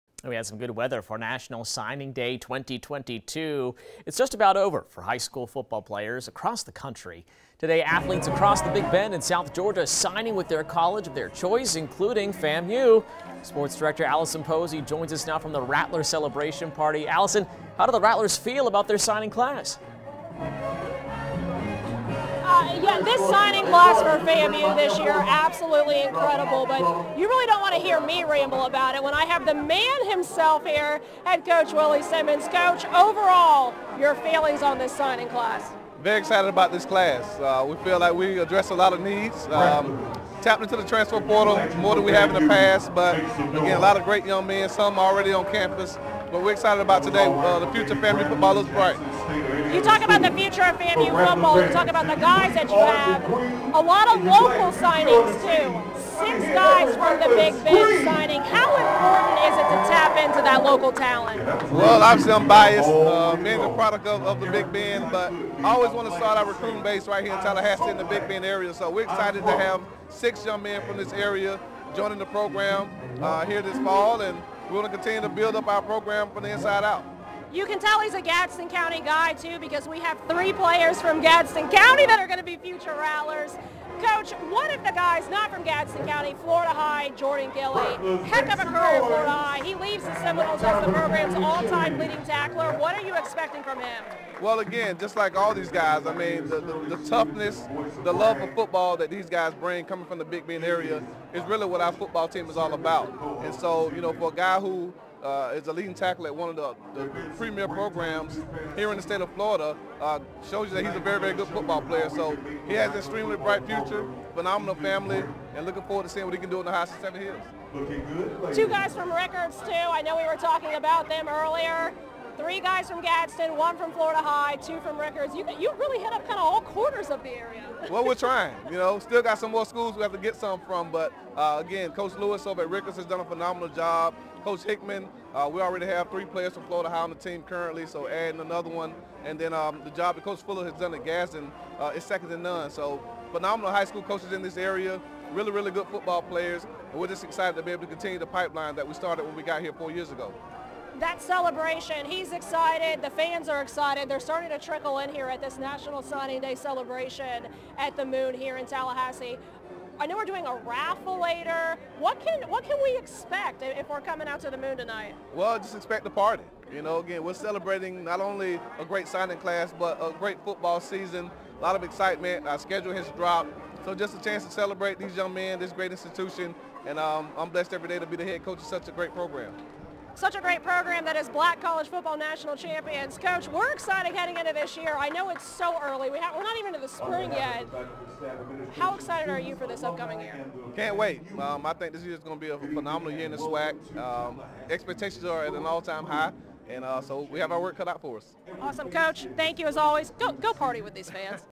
Regular signing period 2022: Student-athletes sign to join Florida A&M football team
Wednesday is the first day of the 2022 Regular Signing Period for NCAA Division I perspective football student-athletes.